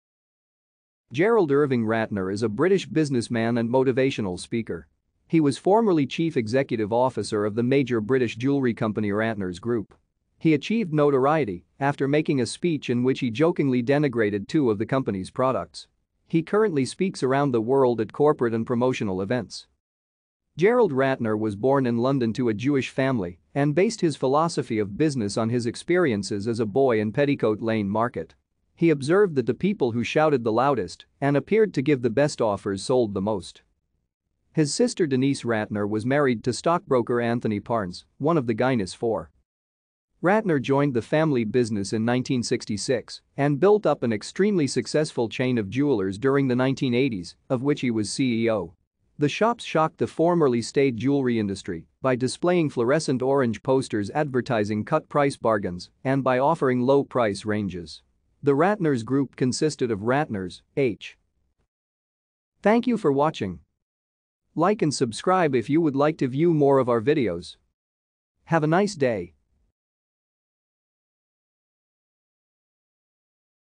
Gerald Ratner
Gerald Irving Ratner  is a British businessman and motivational speaker. He was formerly chief executive officer of the major British jewellery company Ratners Group. He achieved notoriety after making a speech in which he jokingly denigrated two of the company's products. He currently speaks around the world at corporate and promotional events.